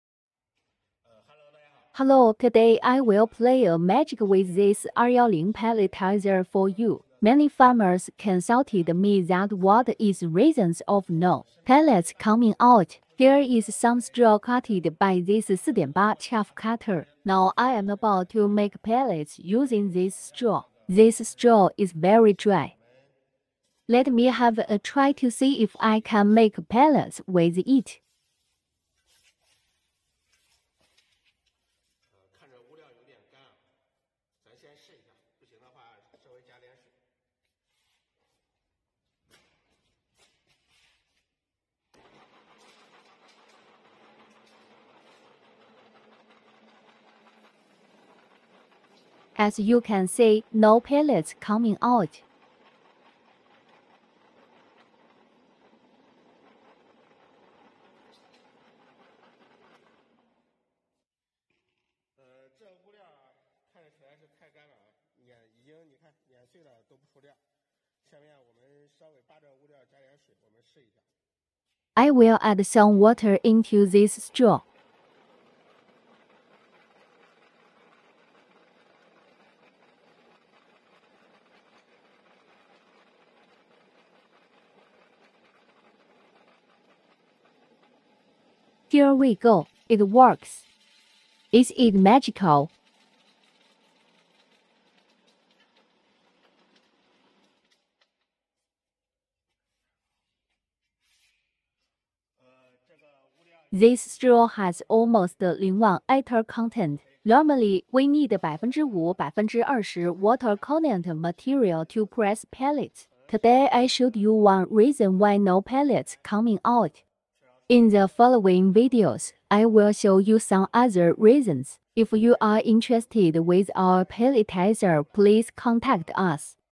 pellet making machine for animal feed, flap die blocks
What is reason why there is no pellets coming out? How to solve this problems?
This video shows you one common reason for this problem.
Sometime, flap die blocks. But there is another problem.
Normally, pellet making machine for animal feed needs 5%-20% water content material. Too dry or too wet material will cause this problem. Material can not be pressed into holes of pelleting equipment die.
If you have the same problem, please make sure that material water content can not beyond 5%-20% for pellet making machine.
I hope this video can help those people who has the same problem with pellet making machine for animal feed.
In the next video, I will show you another reason for this problem.
Hope you are doing well with your pelleting equipment